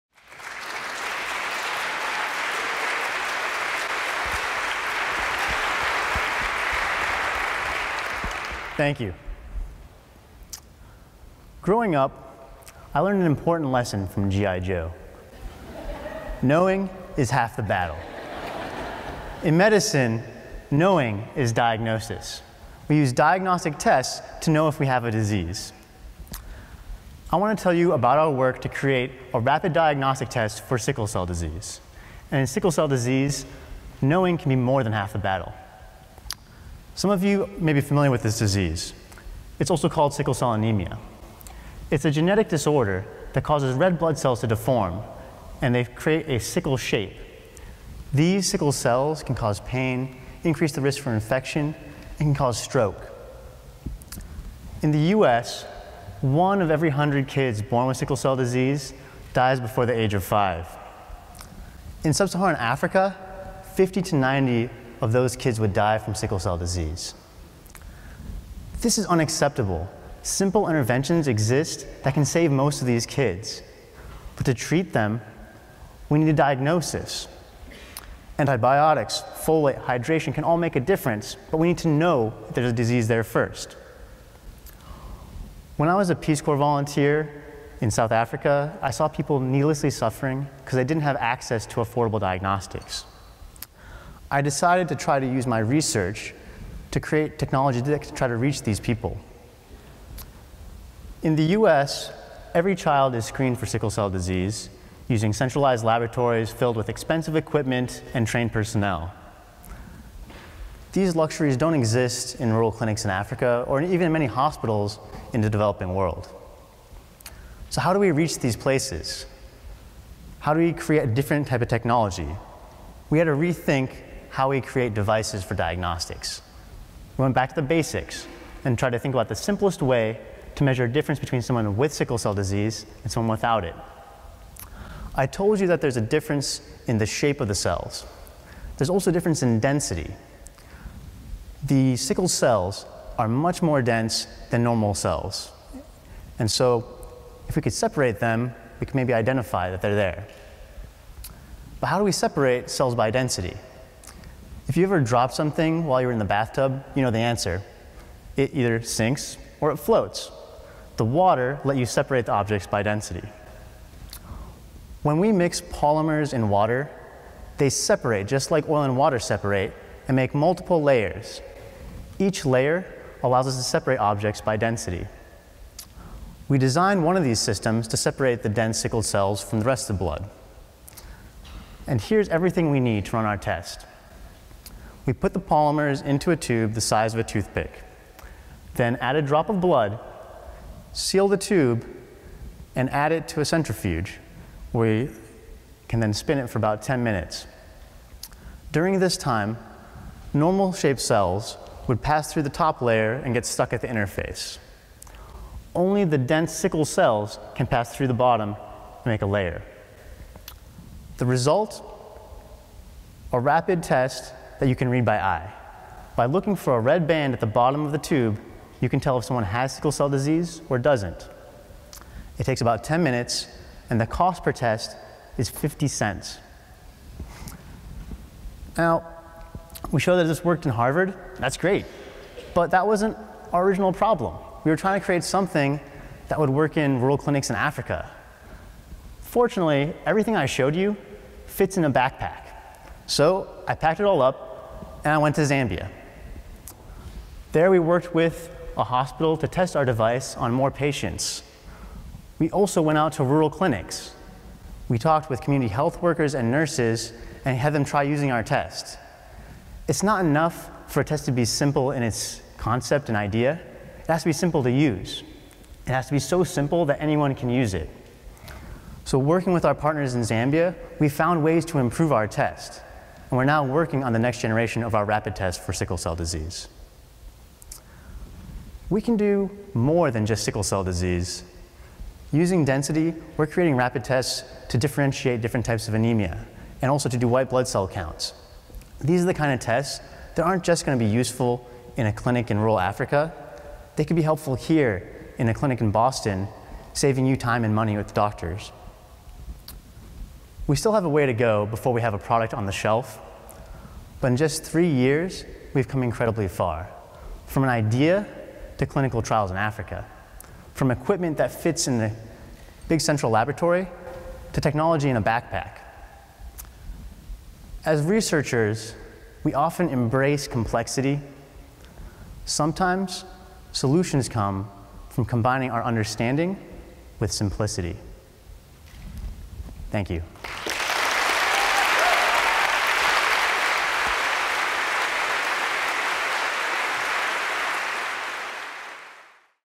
Ashok Kumar | Harvard Horizons Symposium
April 2014: Harvard Horizons Symposium, featuring Ashok Kumar, a PhD candidate in Applied Physics, on "Simple Technology to Tackle Sickle Cell Disease"

Harvard Horizons, an initiative at the Graduate School of Arts and Sciences to celebrate the ideas and innovations of PhD students at Harvard, selected 8 finalists as 2014 Horizon Scholars. Their talks were delivered at the Harvard Horizons Symposium in Sanders Theatre on April 22, 2014.